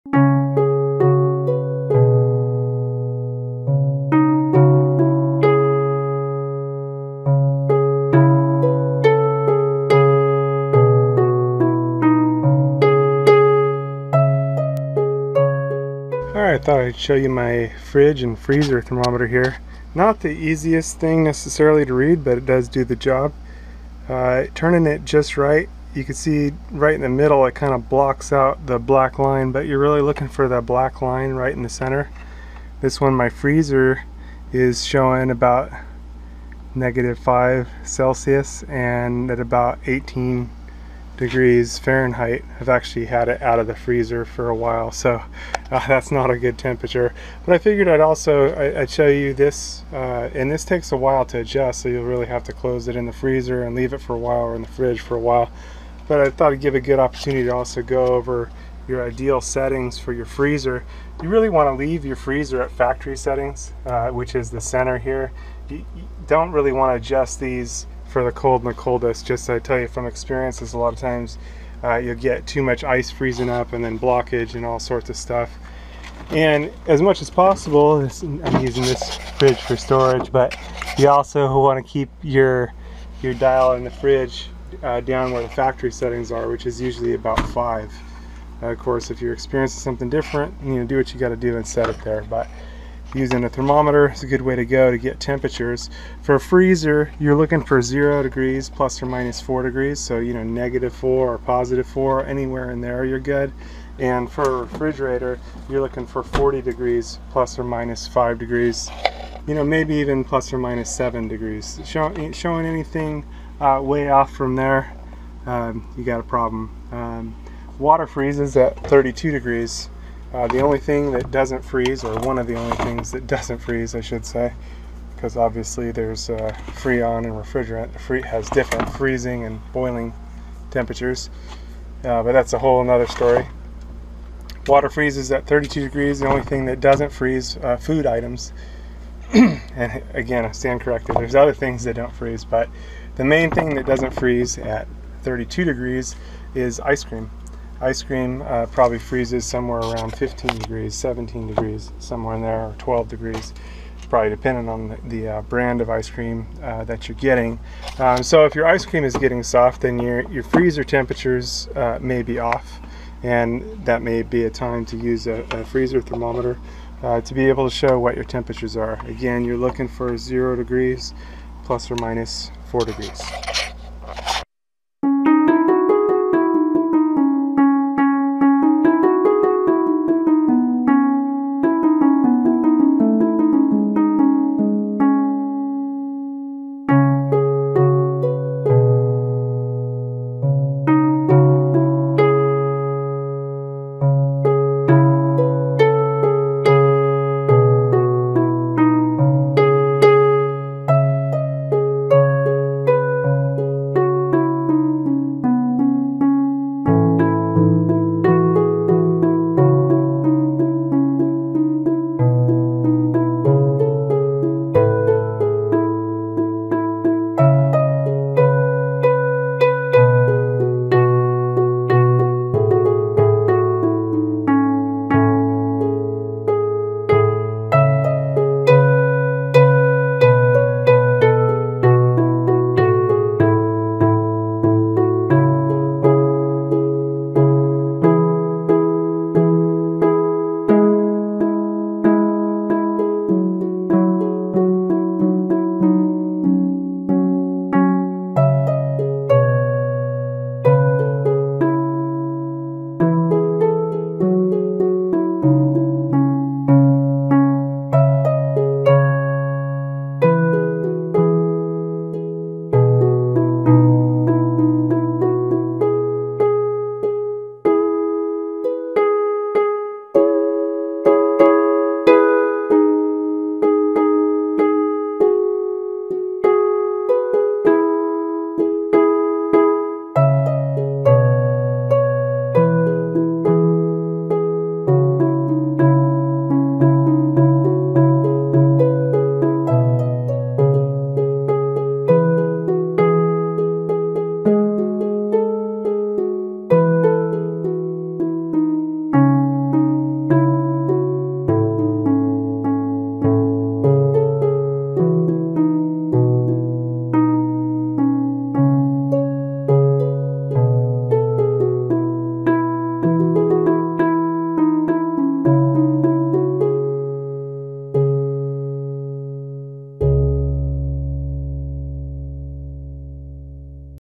Ideal Temperatures For Freezers And Fridges Plus Best Settings For Refrigerators What Should Be At
Kung Fu Maintenance discusses ideal temperatures for freezers and fridges plus best settings for refrigerators what should be at. IceScream YouScream We'll all scream about bad ice cream.
Nice Little Hand Held Drill

I thought I'd show you my fridge and freezer thermometer, here. Not the easiest thing, necessarily, to read, but it does do the job. Turning it just right, you could see, right in the middle, it kind of blocks out the black line. But you're really looking for the black line, right in the center.

This one, in my freezer, is showing about negative 5 Celsius and at about 18 degrees Fahrenheit. I've actually had it out of the freezer for a while, so that's not a good temperature.

But I figured I'd also show you this. And this takes a while to adjust, so you'll really have to close it in the freezer and leave it for a while or in the fridge for a while. But I thought it'd give a good opportunity to also go over your ideal settings for your freezer.

You really want to leave your freezer at factory settings, which is the center here. You don't really want to adjust these for the cold and ant the coldest. I tell you, from experience, a lot of times you'll get too much ice freezing up, and then blockage and all sorts of stuff.

And as much as possible-- I'm using this fridge for storage-- but you also want to keep your dial in the fridge down where the factory settings are, which is usually about 5. Of course, if you're experience is something different, do what you've got to do and set it there.

But using a thermometer is a good way to go to get temperatures. For a freezer, you're looking for 0 degrees plus or minus 4 degrees. So negative 4 or positive 4, anywhere in there, you're good. And for a refrigerator, you're looking for 40 degrees plus or minus 5 degrees, maybe even plus or minus 7 degrees.

Showing anything way off from there, you've got a problem. Water freezes at 32 degrees. The only thing that doesn't freeze-- or one of the only things that doesn't freeze, I should say, because, obviously, freon and refrigerant has different freezing and boiling temperatures. But that's a whole other story.

Water freezes at 32 degrees. The only thing that doesn't freeze, food items. And again, I stand corrected. There's other things that don't freeze. But the main thing that doesn't freeze at 32 degrees is ice cream. Ice cream probably freezes somewhere around 15 degrees, 17 degrees, someone in there, or 12 degrees. It's probably dependent on the brand of ice cream that you're getting.

So if your ice cream is getting soft, then your freezer temperatures may be off. And that may be a time to use a freezer thermometer to be able to show what your temperatures are. Again, you're looking for 0 degrees plus or minus 4 degrees.